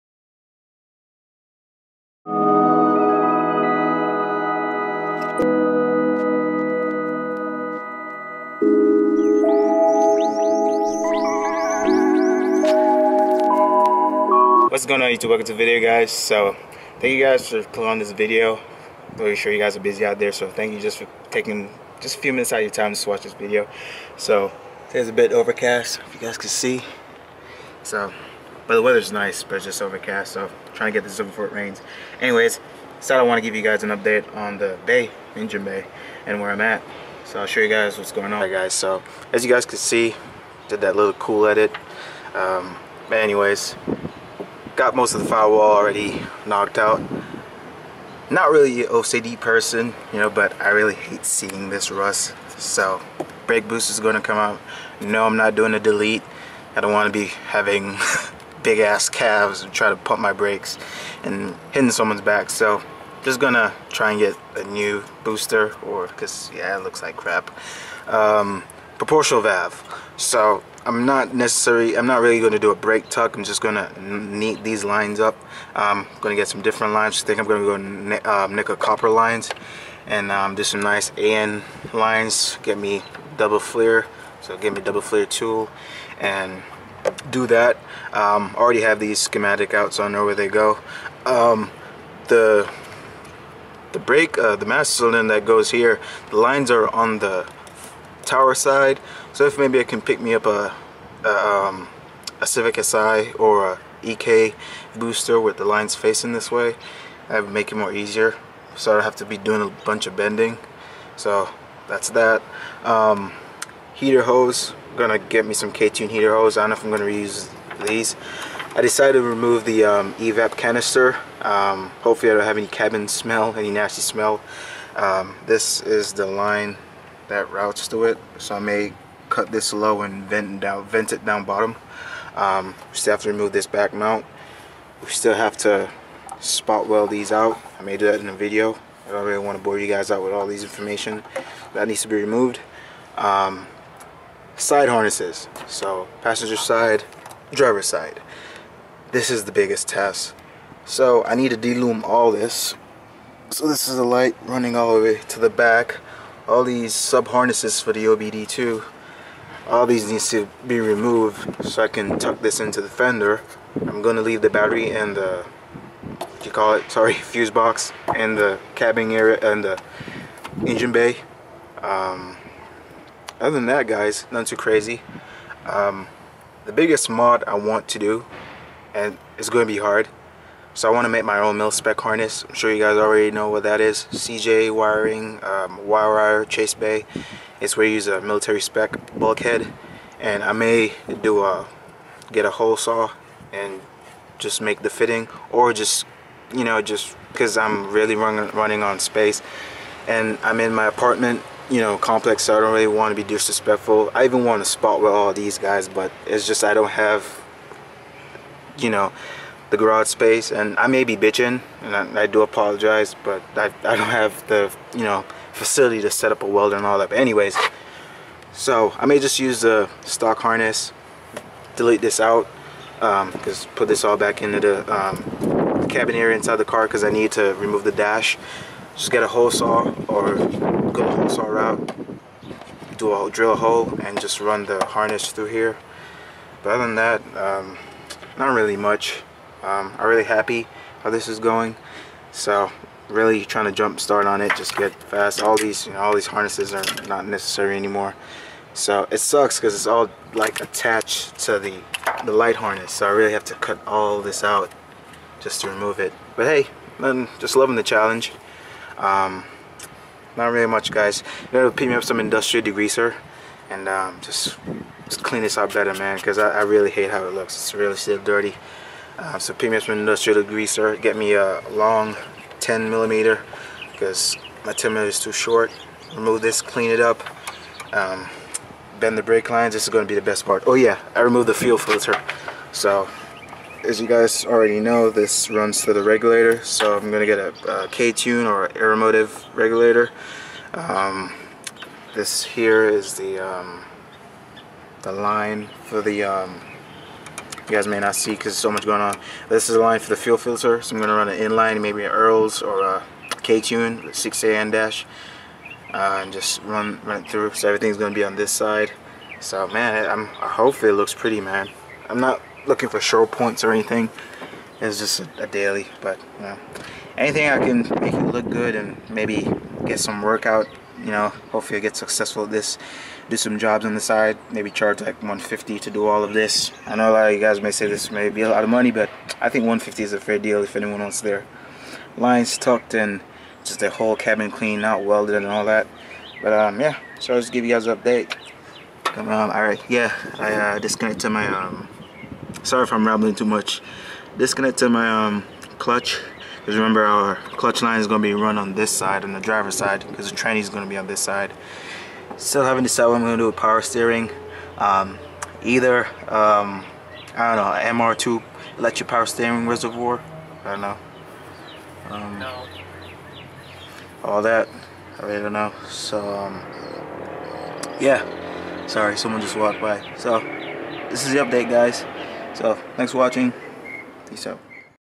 K20 SWAP UPDATE | INTEGRA ENGINE BAY TUCK PREP
Giving you guys an update on the engine bay. I cleaned up most of the firewall and preppring the bay for cleaning an tucking.